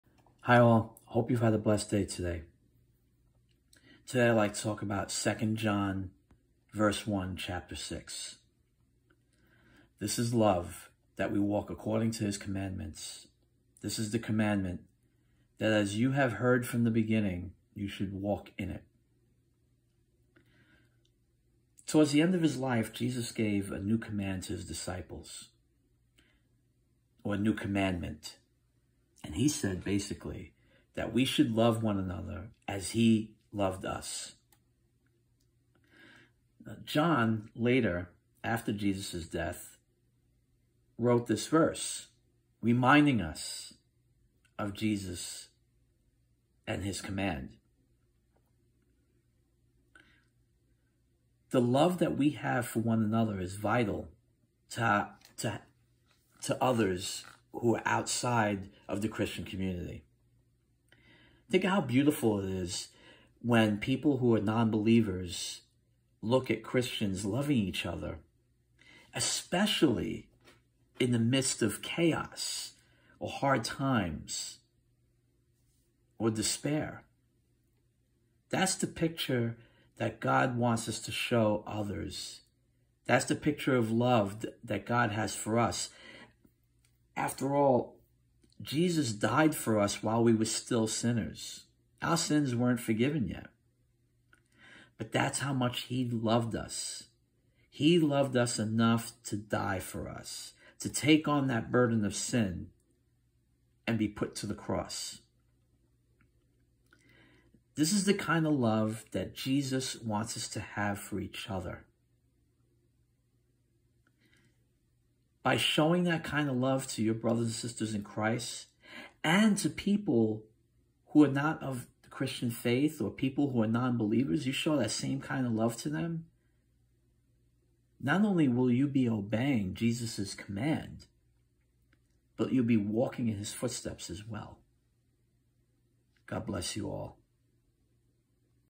2 John 1:6
Jesus gave us a command to love one another as he loved us. This is the example we should be showing people who are nonbelievers. When you show love to everyone around you, you are obeying Jesus’s command and walking in his footsteps. ￼